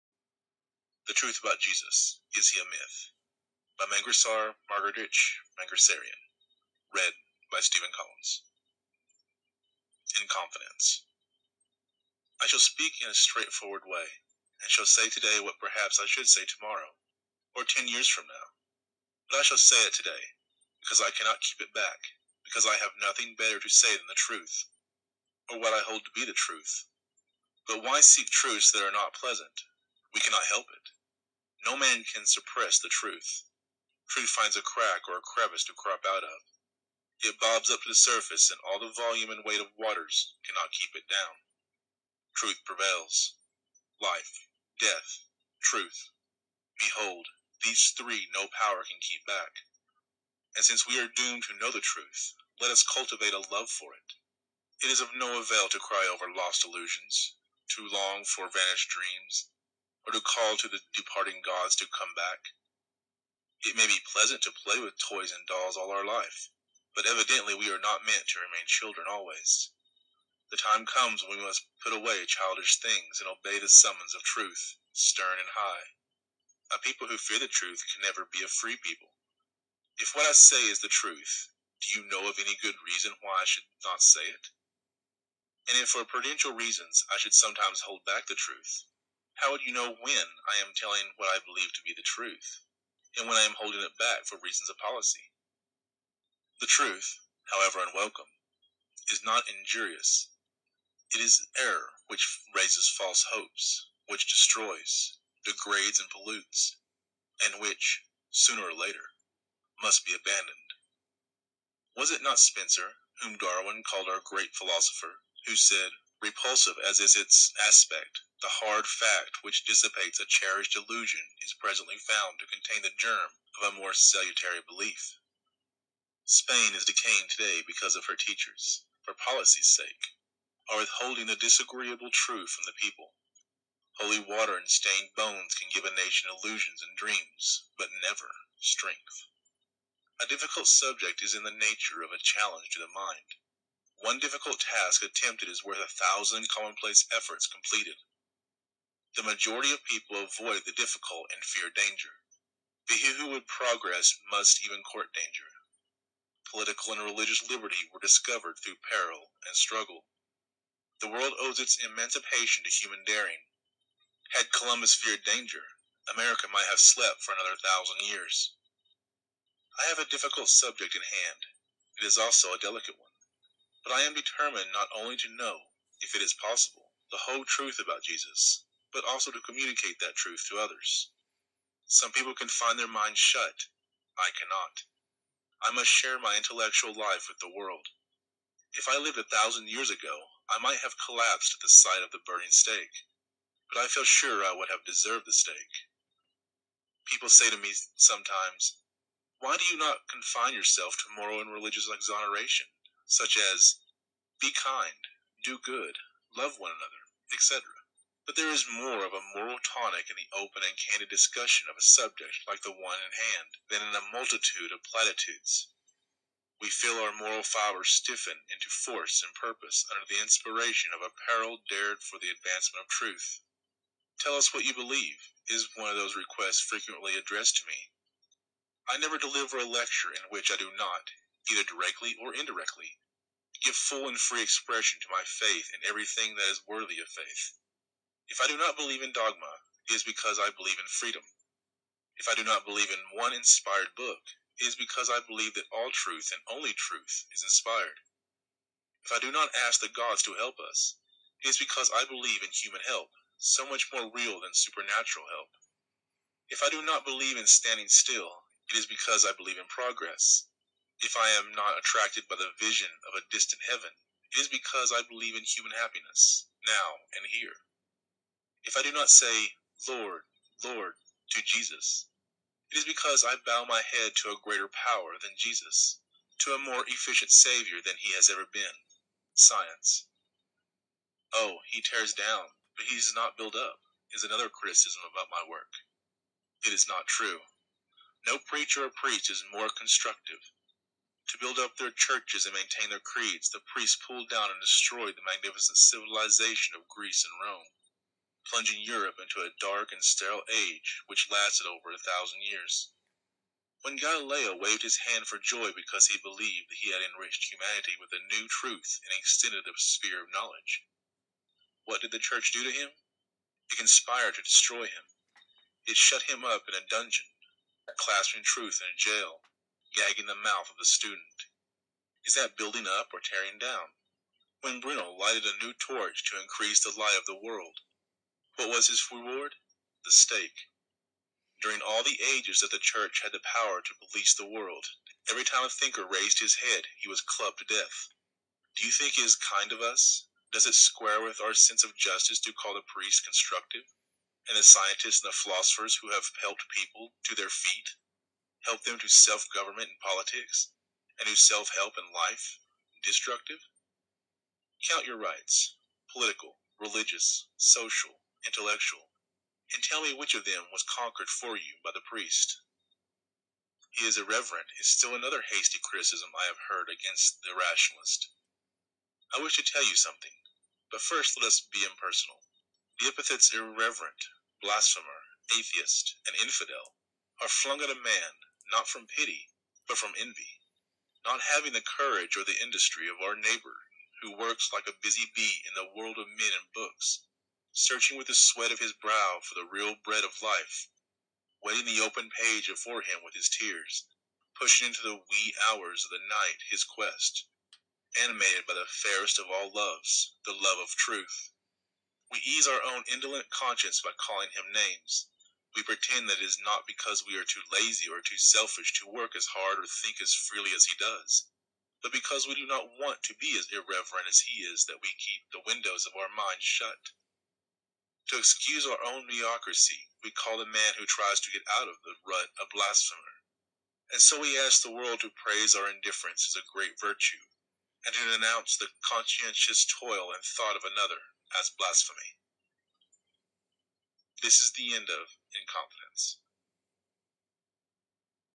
The Truth About Jesus, by M.M. Mangasarian -- In Confidence
Chapter  2
The following work offers in book form the series of studies on the question of the historicity of Jesus, presented from time to time before the Independent Religious Society in Orchestra Hall, Chicago, 1909.

No effort has been made to change the manner of the spoken, into the more regular form of the written, word. Summary by M.M. Mangasarian (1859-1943)

Read for LibriVox.org by Steven Collins. This recording is in the public domain.